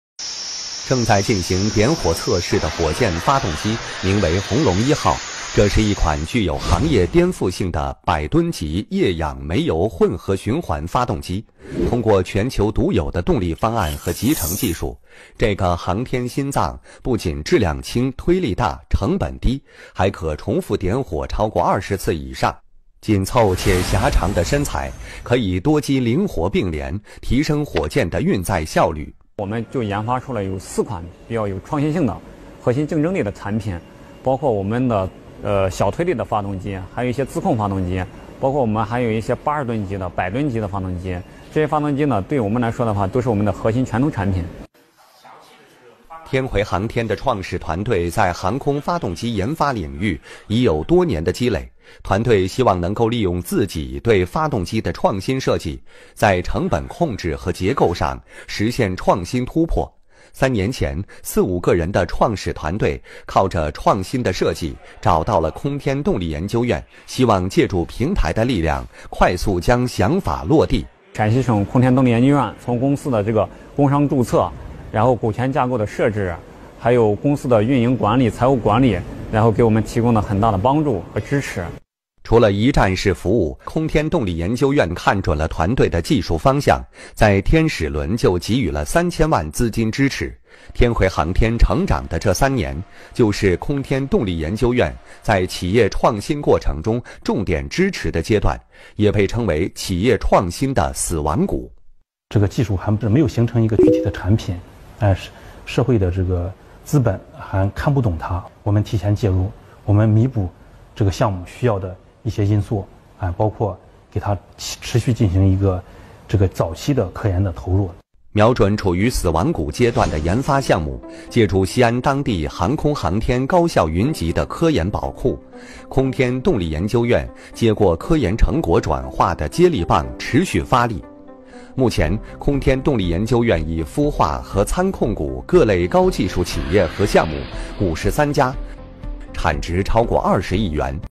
国产火箭发动机可重复点火20次以上
在西安科创中心，百吨级液氧煤油混合循环发动机“红龙一号”正在点火测试。这款具有行业颠覆性的航天“心脏”不仅质量轻、推力大、成本低，还可重复点火超过20次以上。紧凑且狭长的身材，可以多机灵活并联，提升火箭的运载效率。